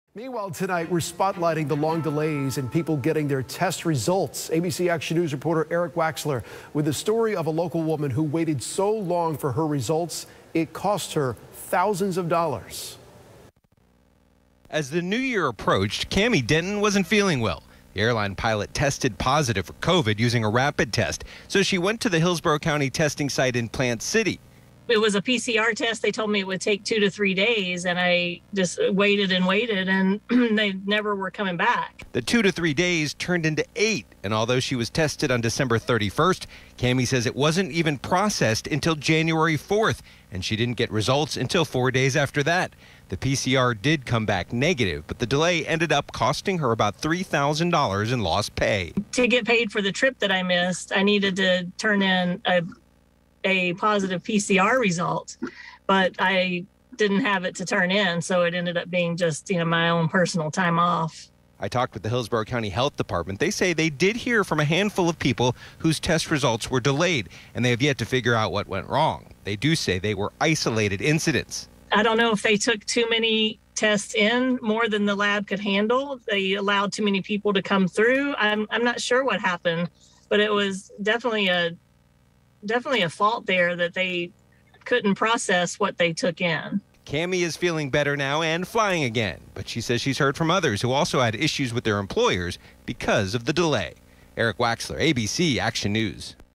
Long delays in COVID-19 test results causing people issues
We're spotlighting the long delays in people getting their test results. ABC Action News reporter Erik Waxler shares the story of a woman who waited so long for results that it cost her thousands of dollars.